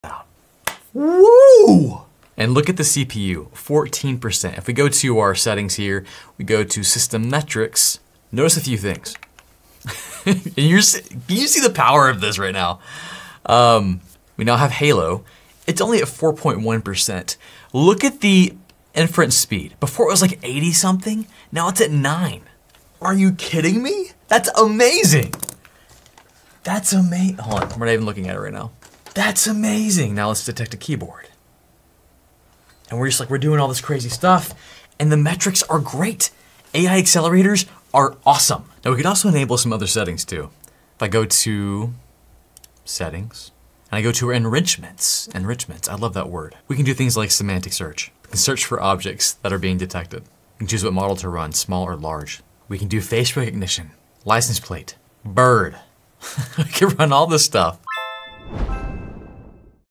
Insane AI Speed Boost! 🚀 Raspberry Pi + Halo Reveal
Can a Raspberry Pi handle real-time AI? Watch the moment the Hailo-8 AI Accelerator (Halo HAT) turns the Raspberry Pi 5 into an Edge AI beast! 🤯

We saw a massive performance reveal: CPU usage dropped to just 14% while the AI module handled the heavy lifting at only 4.1% load. The most shocking part? Inference speed jumped from 80ms down to just 9ms! ⚡️

With this setup, we unlocked advanced "Enrichments" like: ✅ Real-time Object Detection (Keyboards, Birds, & more!) ✅ High-speed Semantic Search ✅ Face Recognition & License Plate Reading ✅ Local, Private AI Processing

Is this the ultimate DIY AI setup for 2025? If you're building robotics, smart home security, or edge computing projects, the Raspberry Pi AI Kit is a game-changer.

Drop a comment: What would you build with 26 TOPS of power on a Pi? 👇